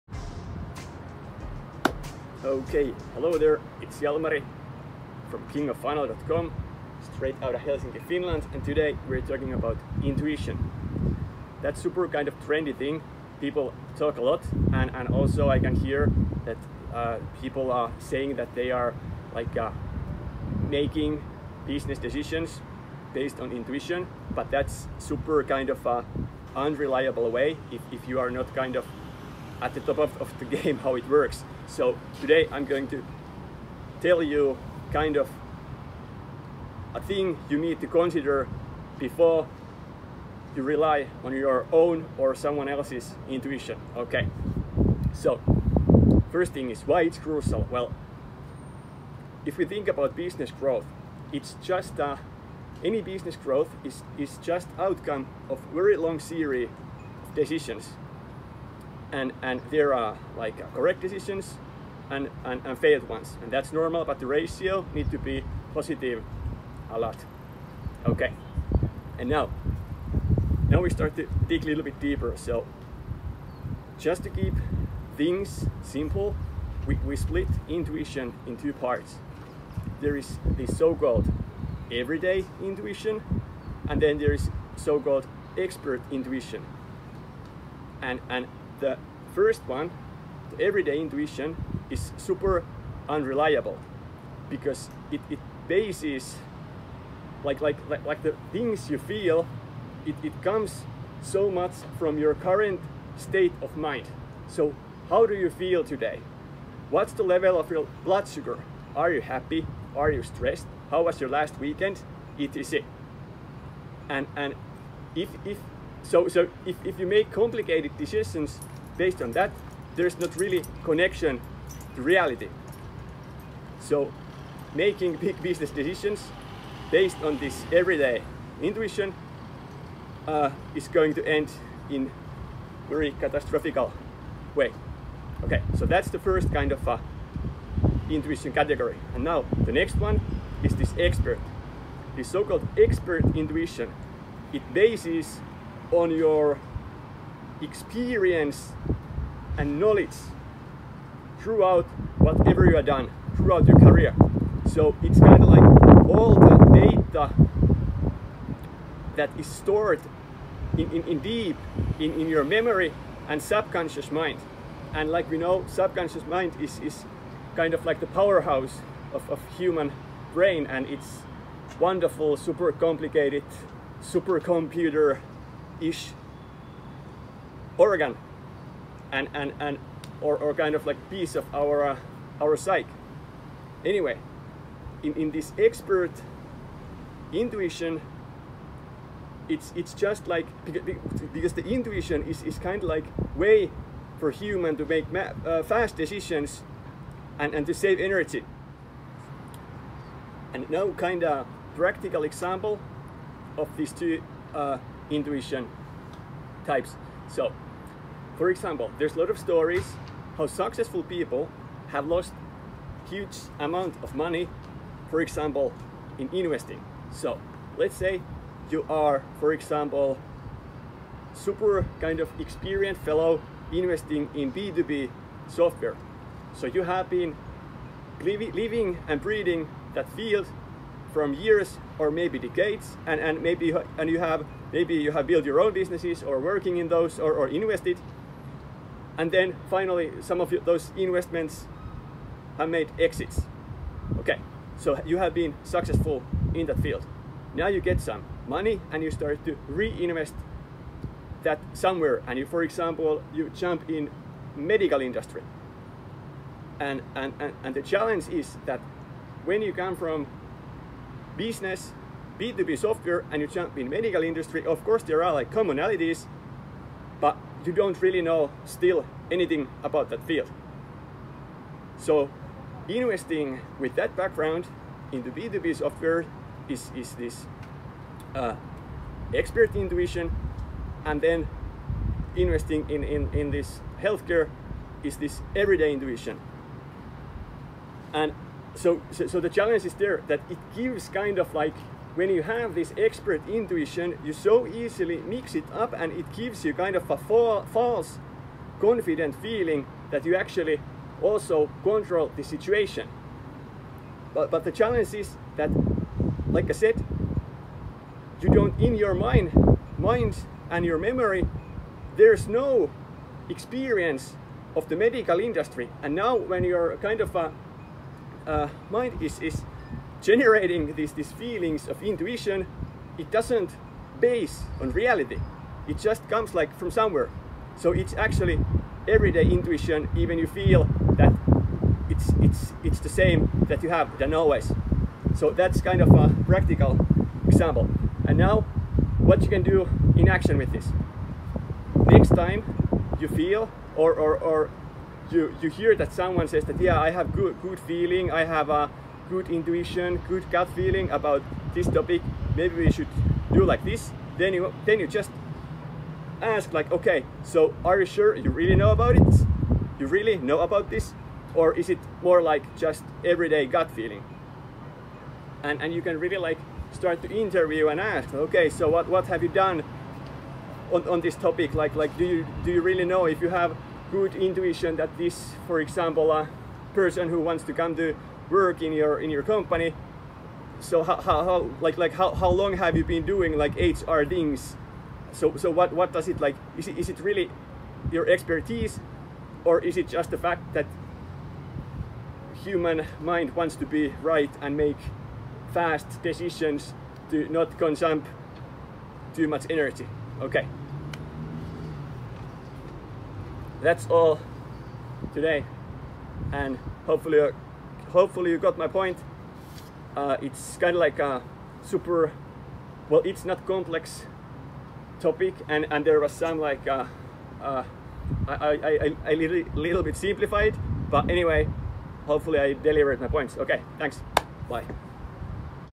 Intuition & Growth? Growth is a result of a long series of CORRECT DECISIONS.
Intuition & Growth? 👽

Growth is a result of a long series of CORRECT DECISIONS.

People say they decided something because of strong intuition...

That’s a risky way to go if don't know how intuition works.

There is two kinds of intuition (slight simplification):

✅ 1) Every-day intuition

This is a shaky “gut feeling”,

It origins from your current MOOD... (are you happy, sad, what’s ur levels of blood-sugar, did you sleep well last knight etc…?

There’s no much connection to reality if you need to analyze and decide complex cause-and-effect relationships.

✅ 2) The expert intuition

It’s based on all the experiences stored in you your memory. The unconscious mind is a damn power-house of understanding.

If you really know you shit, there’s a chance that your gut feeling (intuition) may drop you some solid insights.

But what’s the risk if you mix up these two?

See more about this topic in the video.